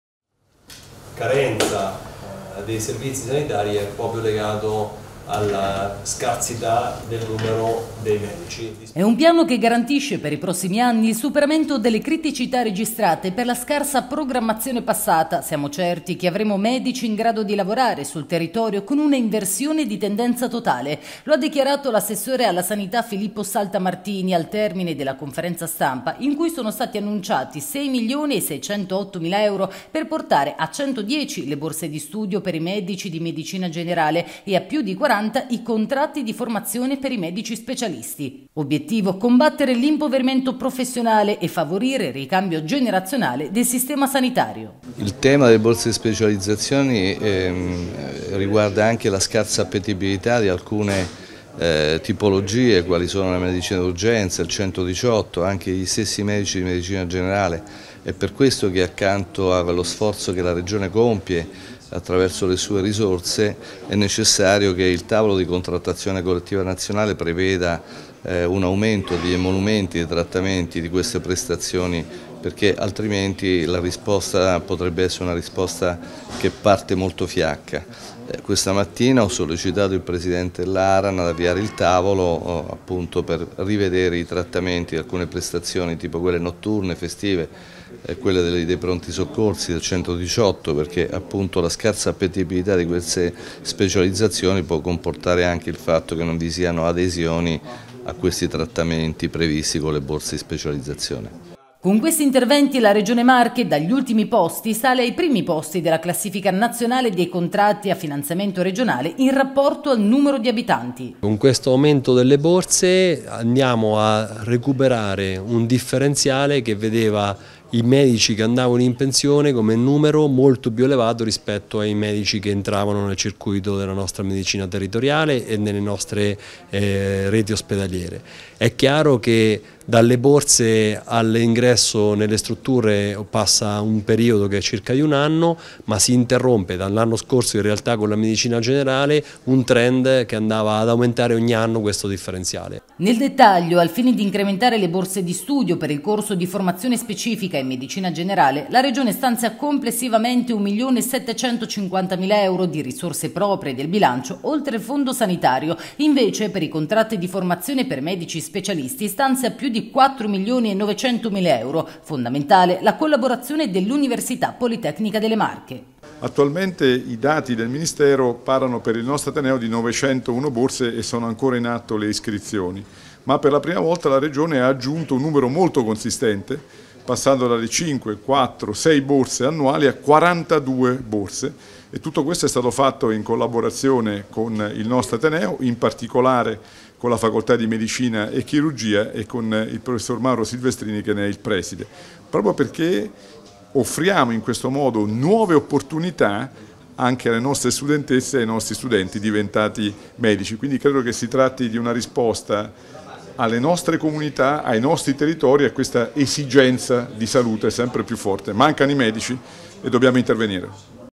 Medici di base e specialisti: Regione stanzia 6,6 milioni
Carenza di medici: la Regione stanzia oltre 6 milioni di euro per le borse di studio e i contratti di formazione. Per la giunta Acquaroli si tratta di una “svolta che supera le criticità lasciate da una scarsa programmazione passata”.